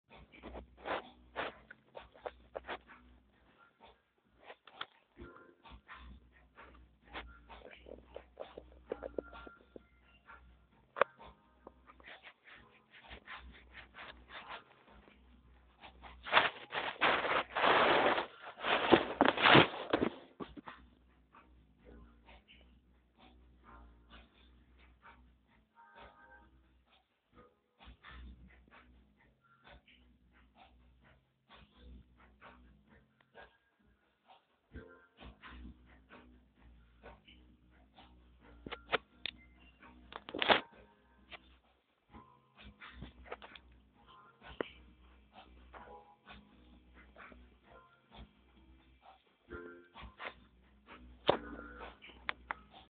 Suri ao ataque
suri the cat, fighting with the camera.